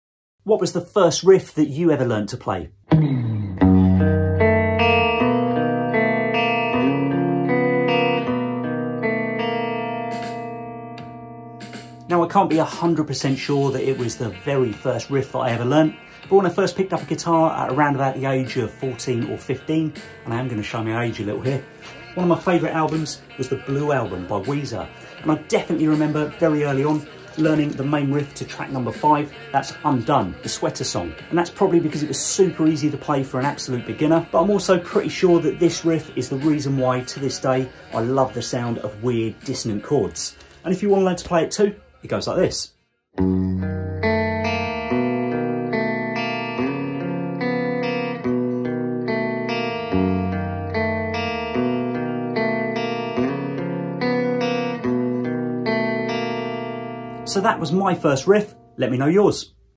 What Was The First Guitar Riff You Learnt To Play?!
Every guitar player knows the first song they learnt on electric guitar, right?! Here's Sam's, let us know yours in the comments! 🤘